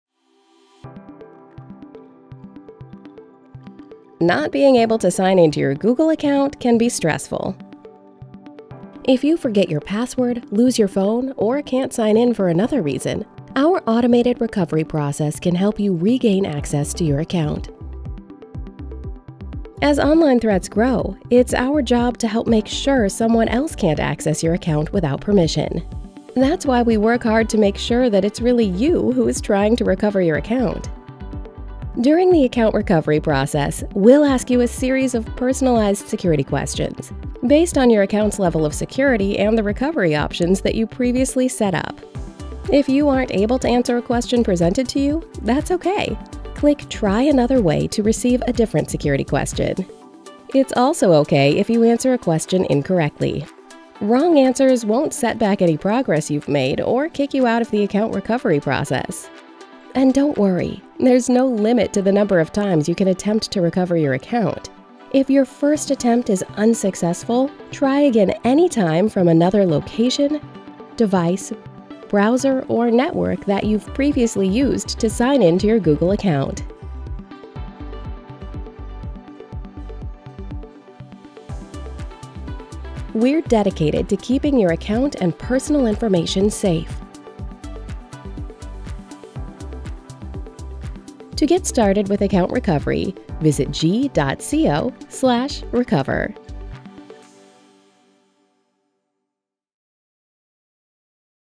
How to recover access to your Google Account
Can’t sign in to your Google Account? Watch this video to learn how to recover it. With Google’s automated recovery process, you can retrieve your Google Account using any device, location, browser, or network that you previously used to access your account. This process helps keep  your account safe from online threats and unauthorized access.